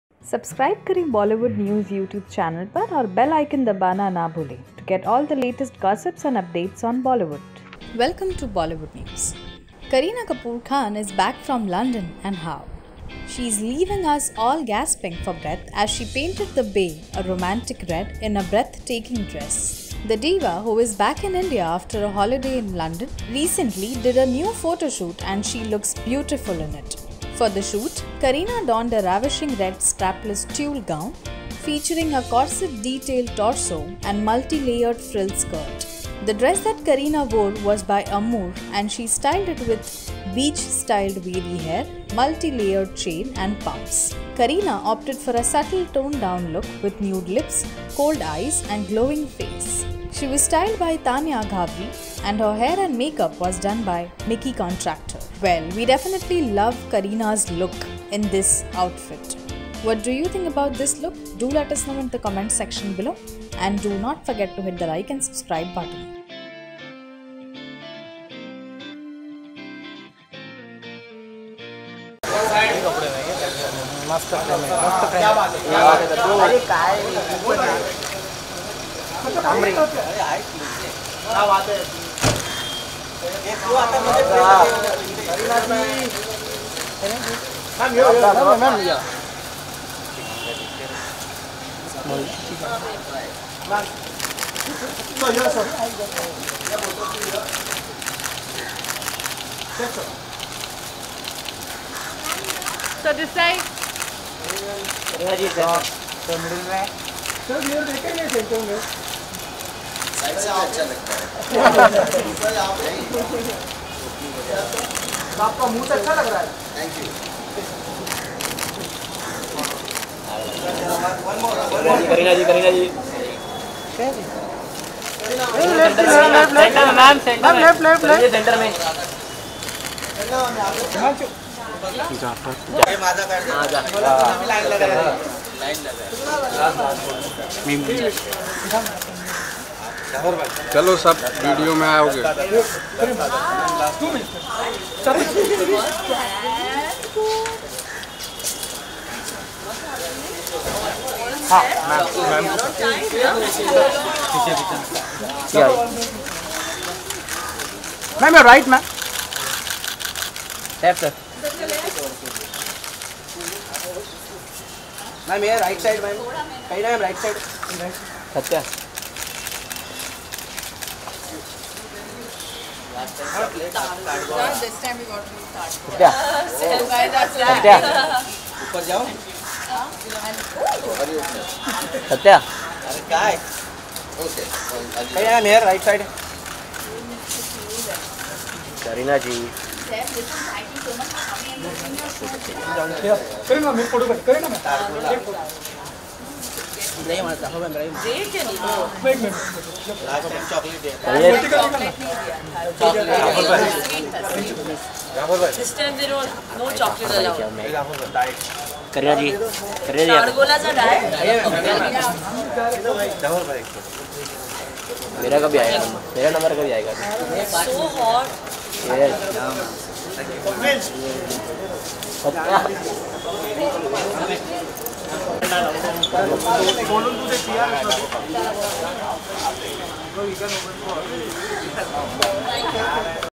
Kareena Kapoor Khan PREGNANT Again and Flaunts her Baby Bump at Dance India Dance Set
Kareena Kapoor Khan PREGNANT Again and Flaunts her Baby Bump at Dance India Dance